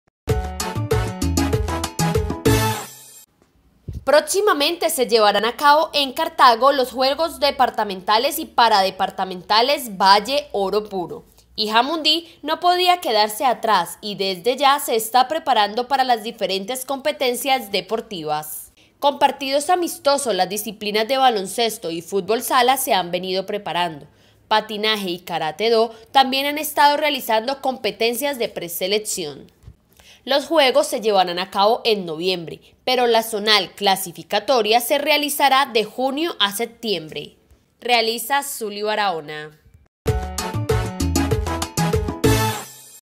JUEGOS DEPARTAMENTALES 2017
Jamundí se alista para las competencias deportivas departamentales.